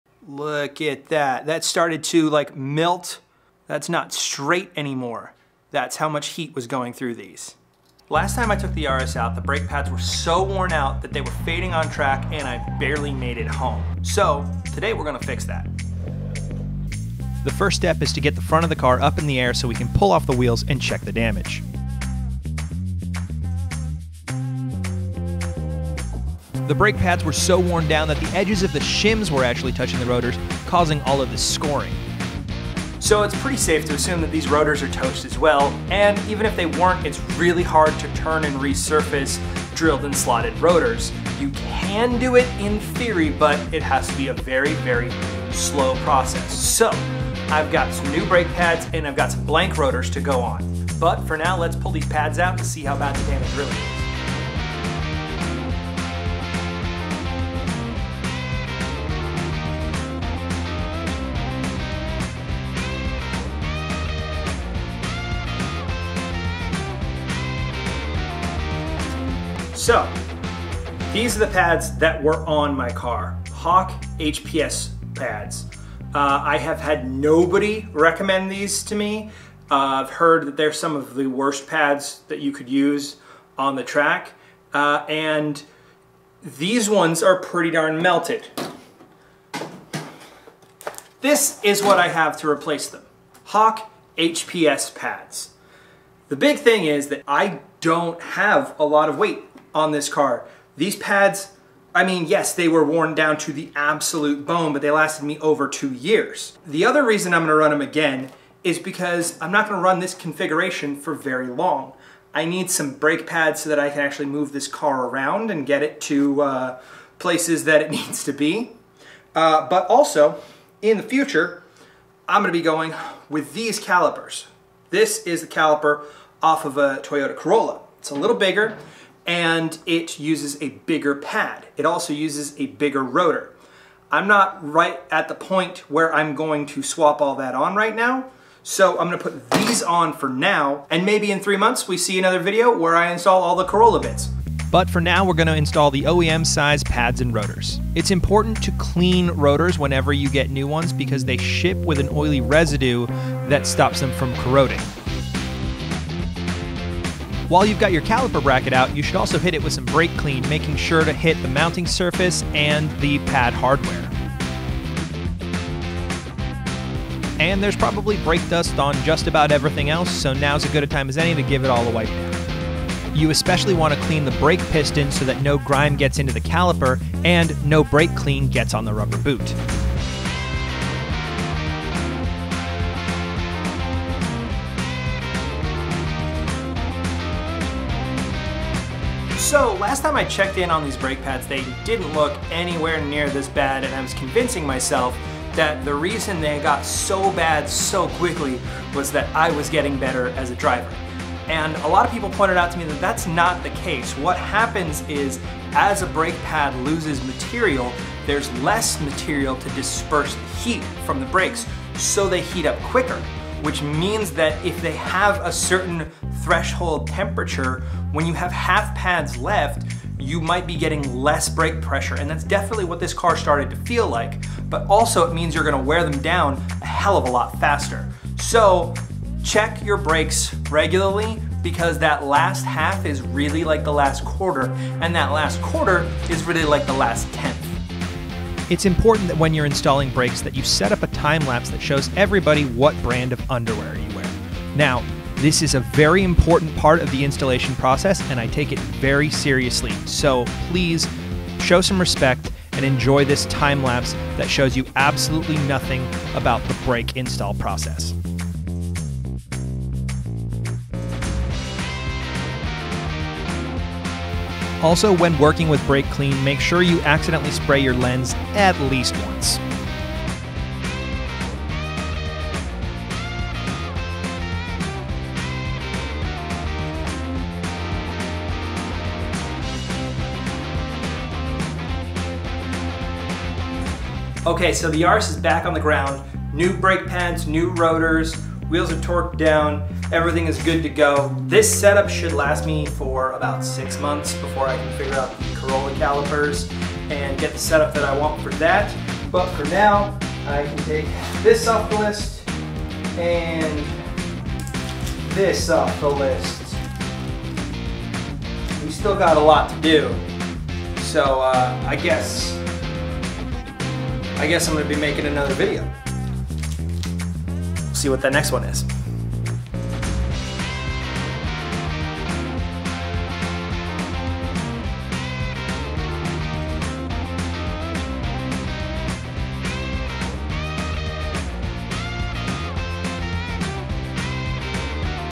I melted my brakes
the 2000lb yaris really isnt hard on brakes, but 2 years of track days finally took their toll.

Additional on Track Photos by CaliPhotography and Nic.tellez

Follow these cars on instagram @StartingStock